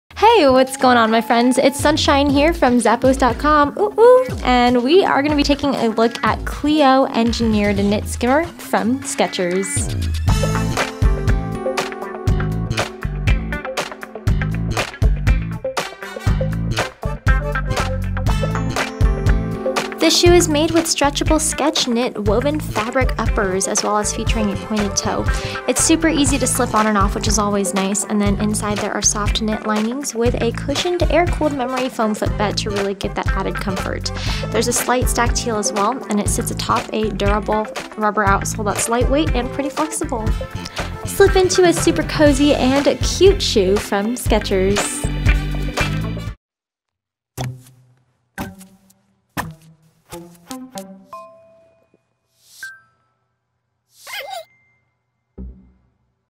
SKECHERS Cleo - Engineered Knit Skimmer SKU:8891286
Freshen up your style with the fashionable Cleo - Engineered Knit Skimmer flat from SKECHERS®.
Stretchable Skech Knit woven fabric uppers with a pointy toe.
Easy slip-on style flat.
Soft knit lining.
Cushioned Air-Cooled memory foam footbed for added comfort.
Flexible rubber outsole.
Imported.